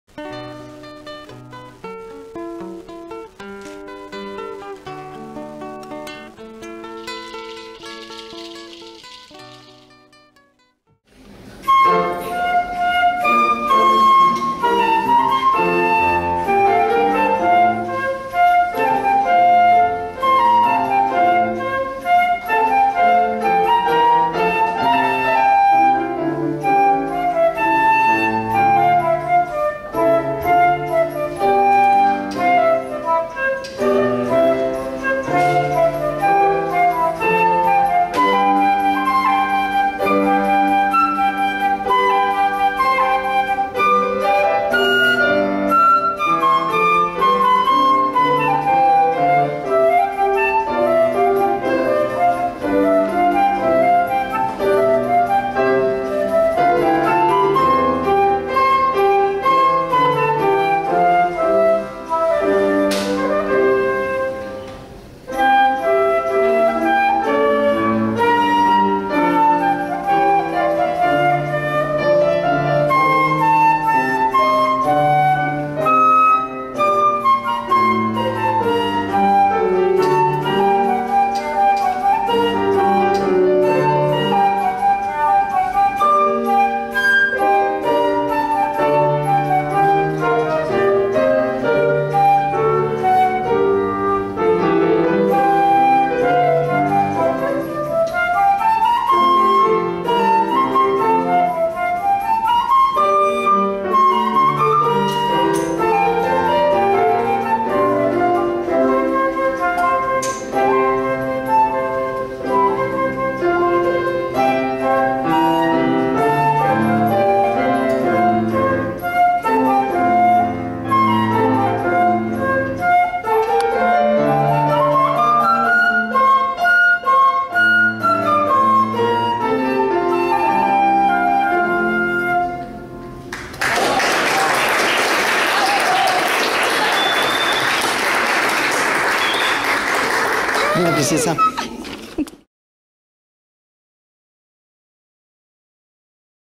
Valentina Cosíos en un certamen de Flauta en el Conservatorio de música.
Este miércoles acompáñanos en el plantón por los desaparecidos y muertes violentas.
En este vídeo Valentina interpretando su flauta.